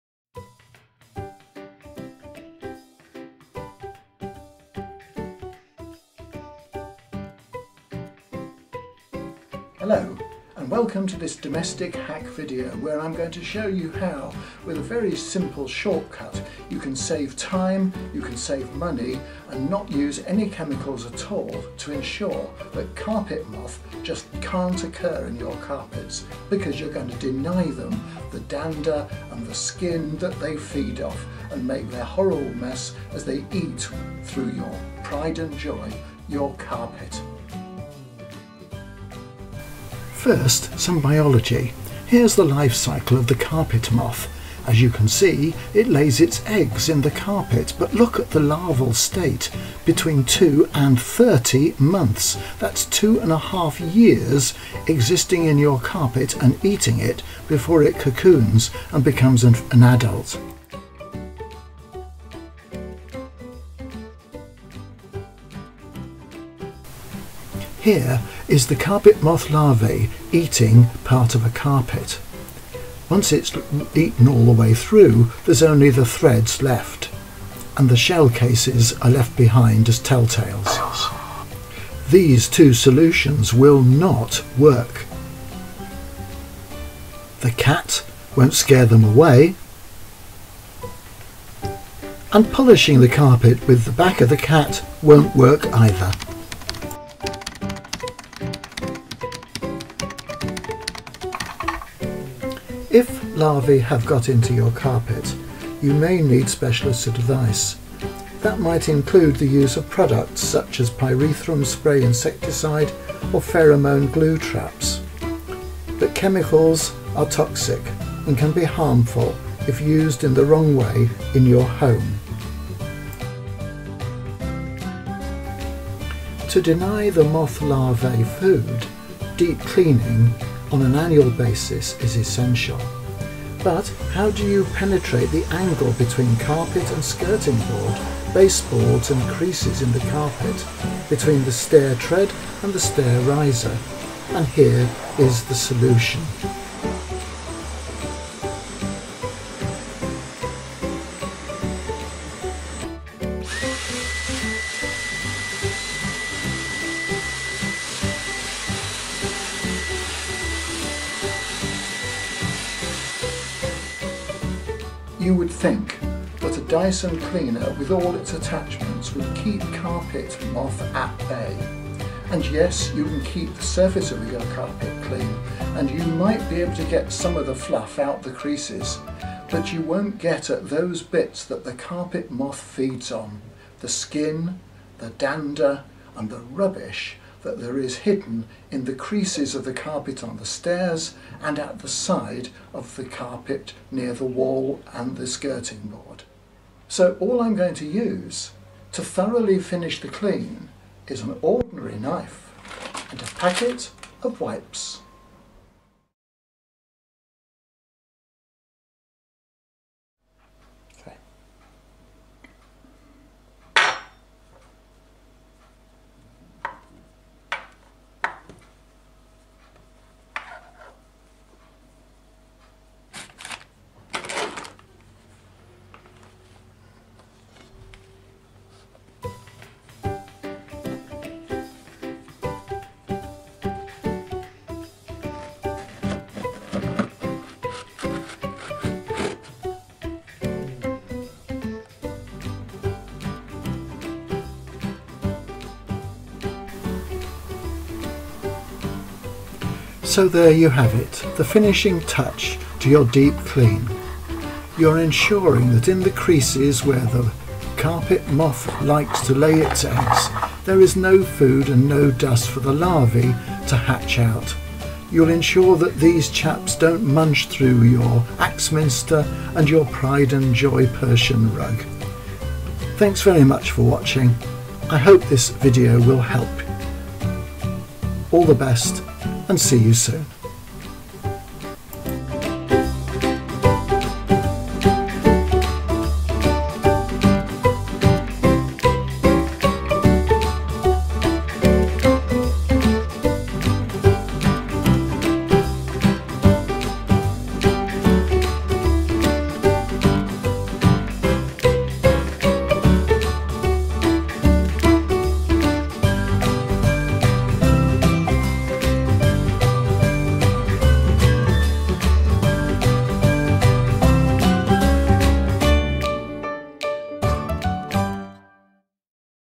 How to prevent Carpet Moth Larvae from eating your carpet - no chemicals - cheap - easy
Carpet Moth Larvae are an expensive and annoying pest. Some homemakers speak of becoming paranoid over ridding their home of them. The solution is not to provide the conditions where they thrive - i.e. very deep and meticulous cleaning. Regular vacuuming is more important than a chemical pest treatment. You don't want it to start.

But how do you deep clean into the angle between carpet and baseboard - skirting board? The solution is in the video!! Please give it a try. We have never had carpet moth or larvae. JSN July 2020